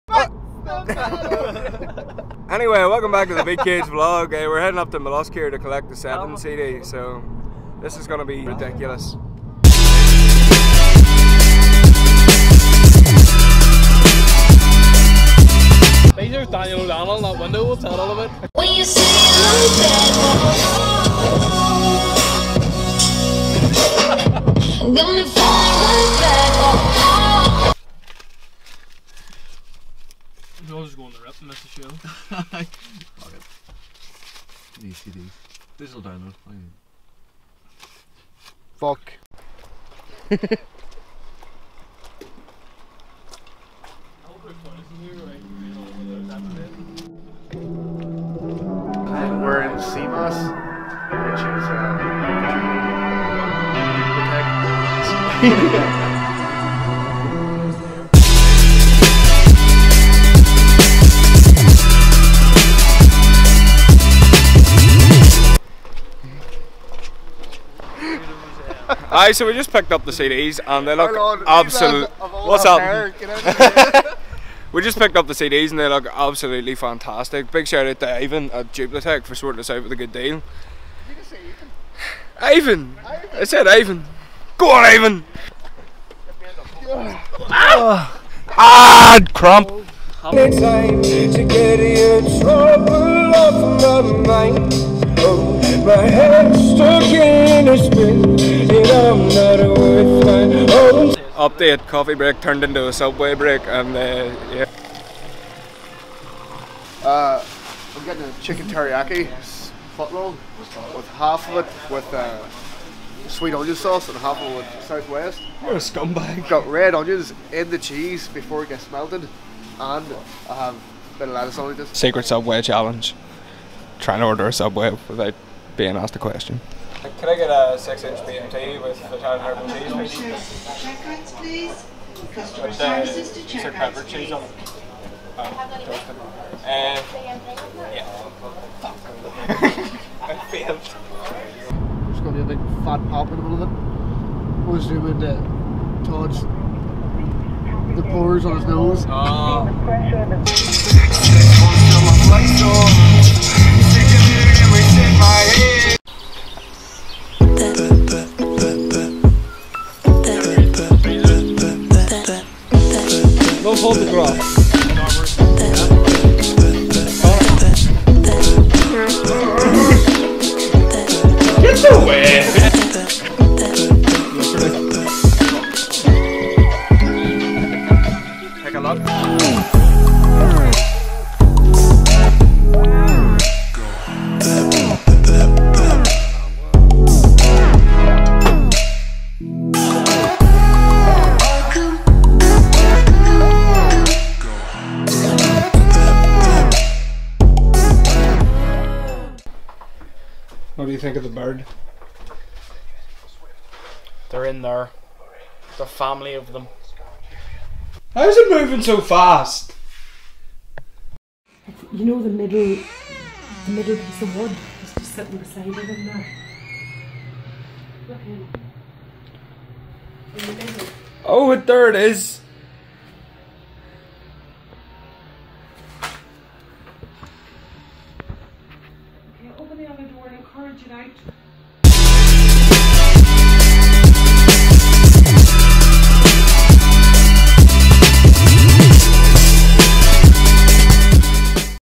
Beatcage Vlog 6 // CD PRINTING & TRAPPED BIRD
Beatcage VLOG's brings you a inside look at what we do along with loads of special features.

In this edition Paul joins "The Seven Project" as they collect their CDs for the big show.